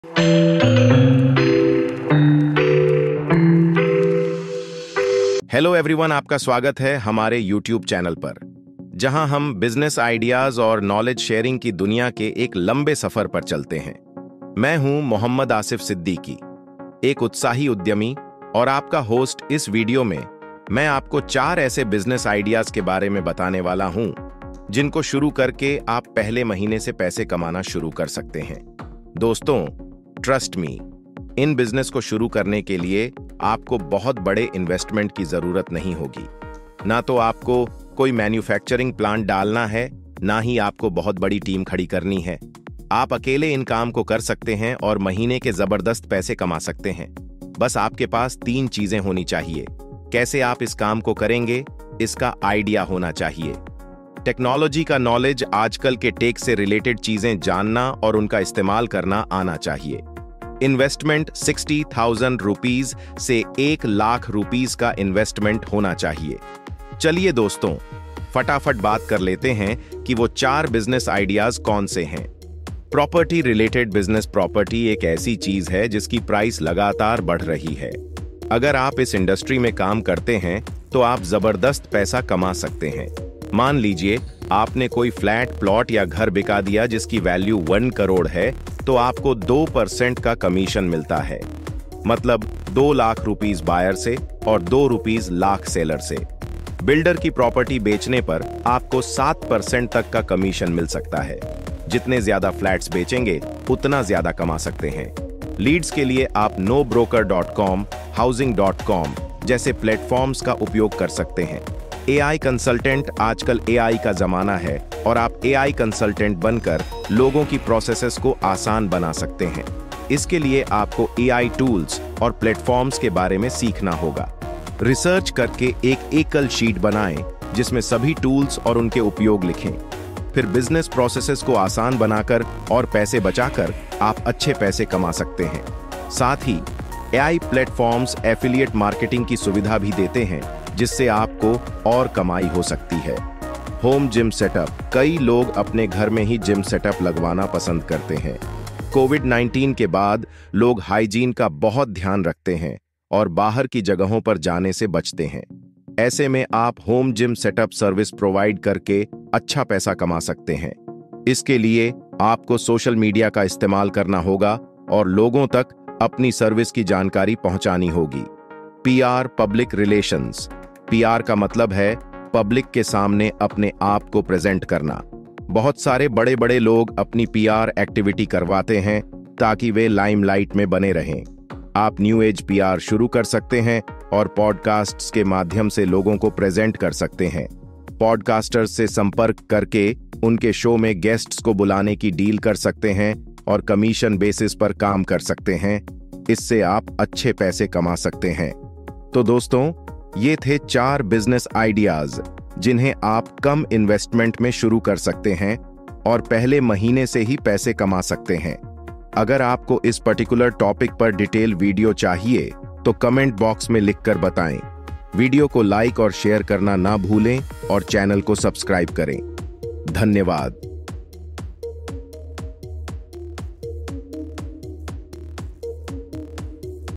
Top 4 Business Ideas
Top 4 Business Ideas for 2024 @dearasifin

AI-Powered Consulting Services: Leverage AI tools to enhance productivity in your niche. Start a business consulting service that helps entrepreneurs integrate AI to streamline operations and boost efficiency.

Healthy Food Delivery to Airports: Develop a healthy food delivery service specifically for airport travelers. Provide nutritious, homemade meals to busy families and health-conscious individuals, ensuring a better travel experience.

Home Organizing and Decluttering Services: Offer professional home organizing services that include decluttering, recycling, and selling unused items. This holistic approach helps clients maintain a tidy, stress-free living space.

AI Chatbots for Niche Markets: Create specialized AI chatbots tailored to specific industries, such as education or healthcare. These chatbots can provide personalized assistance and streamline processes, making services more accessible and efficient.